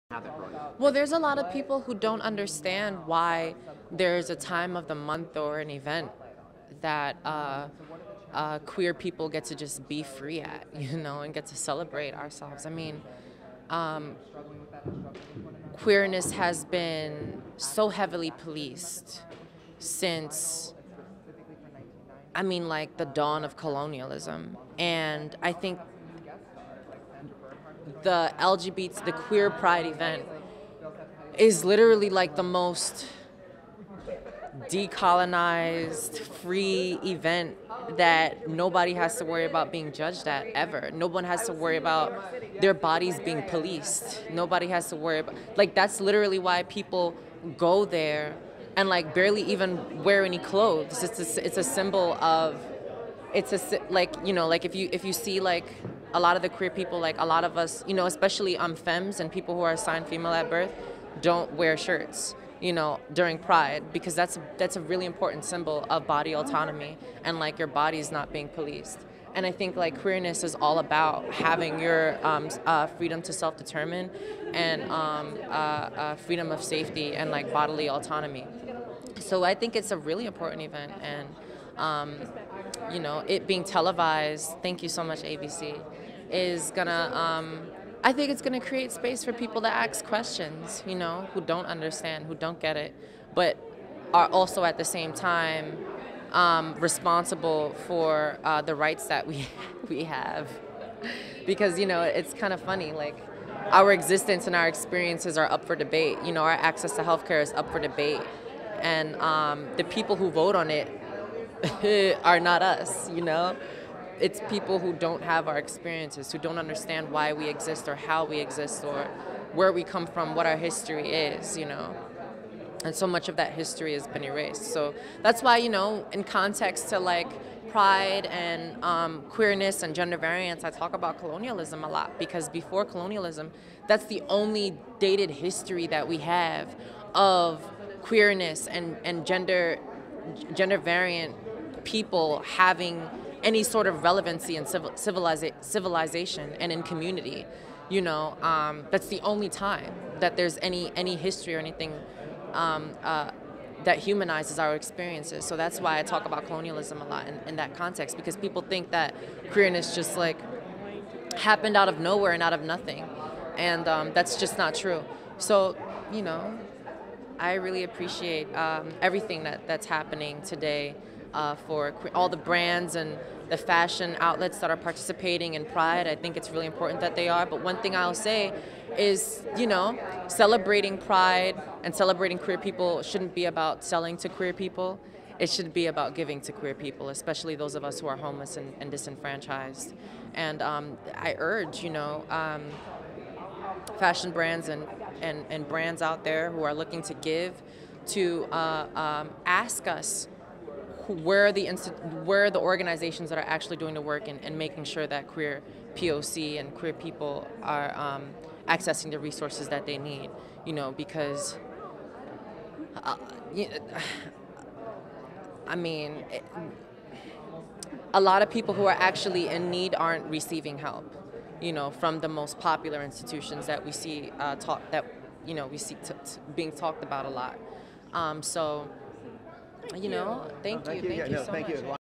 Indya Moore From "Pose" on Why Pride Month Visibility is So Important
Indya Moore from FX's hit show "Pose" shares the importance of televising Pride Month and the freedoms all the celebrations provide those who identify as queer!